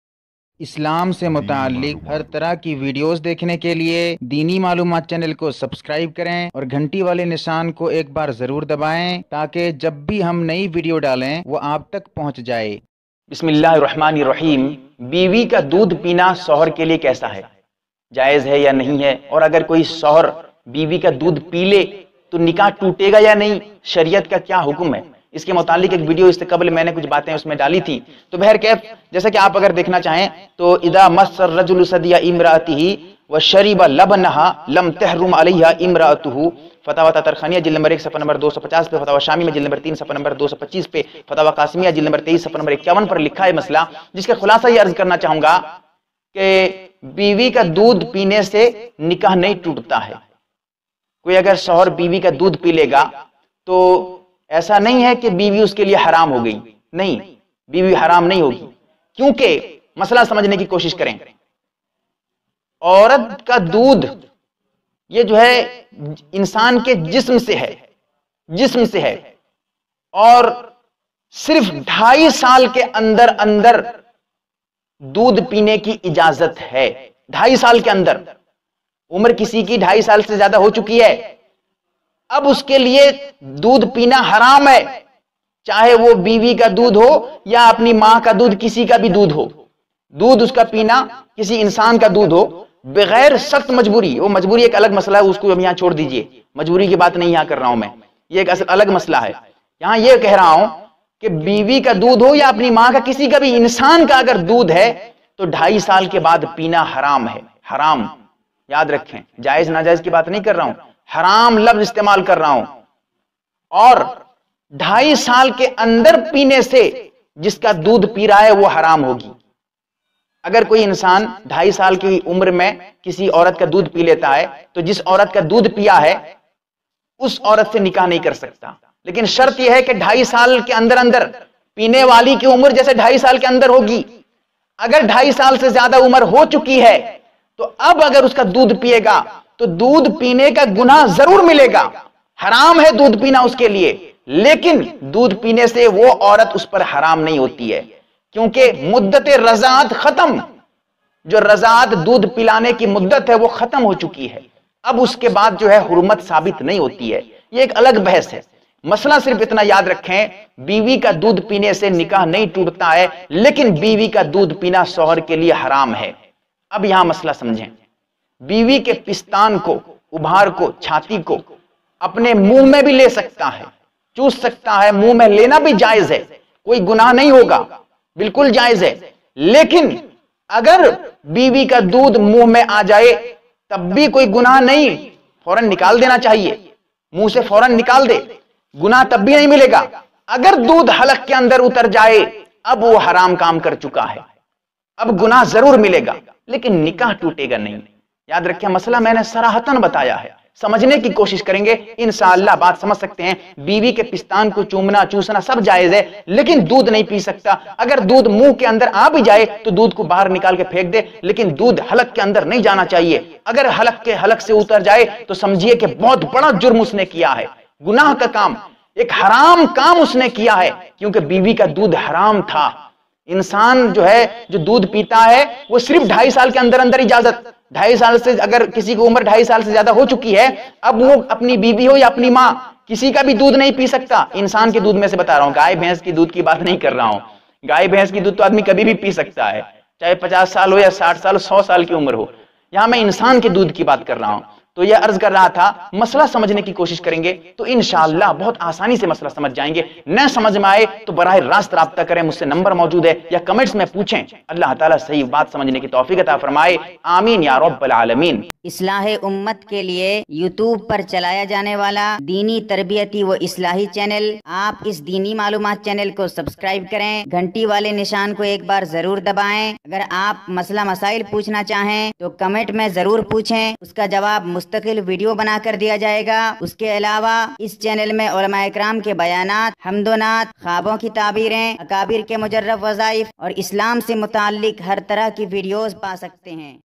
Biwi ke pastan chumna kaisa | kya biwi ka doodh peene se nikah toot jata hai | biwi ka doodh chusna
Biwi doodh peena kaisa hai yani humbistari ke doran shohar biwi ka pastan/seena/ chest choosna or chumna kaisa hai or agar shohar biwi ka doodh peele to kya nikah toot jayega kya hukum hai is video me mukammal tafseel ke saath bahut kuch bataya gaya hai,

Channel by

DEENIMALUMAAT